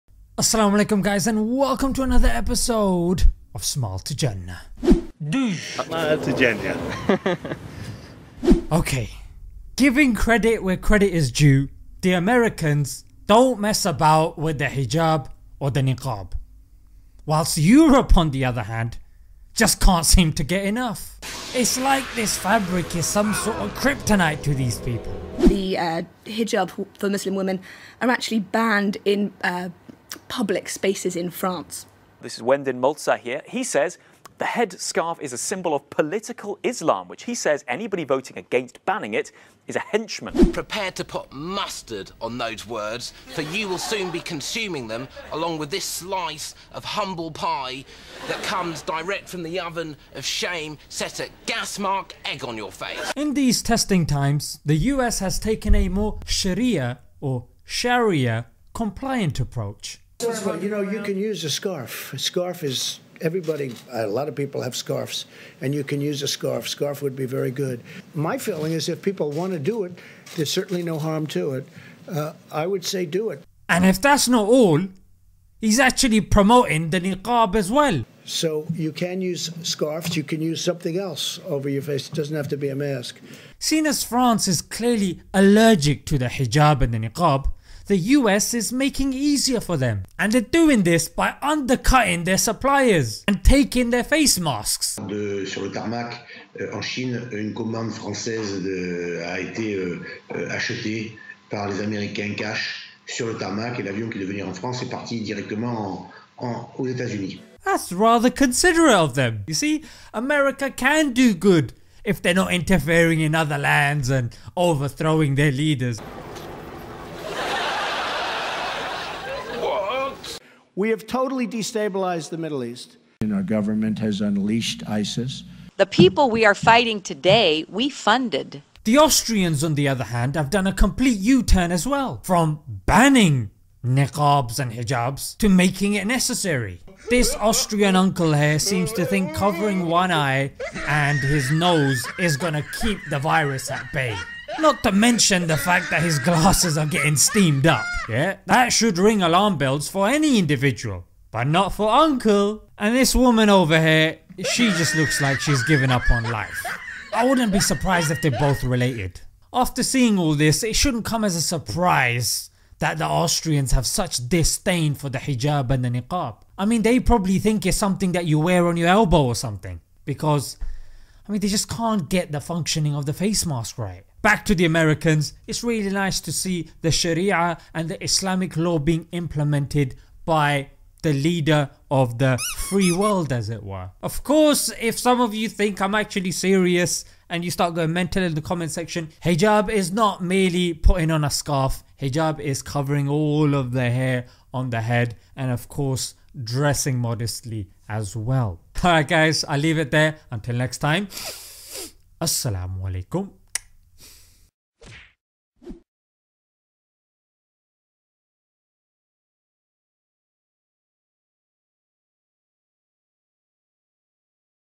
TRUMP TELLS AMERICANS TO WEAR HIJAB?
Who'd have thought it? Trump bigging up the scarf.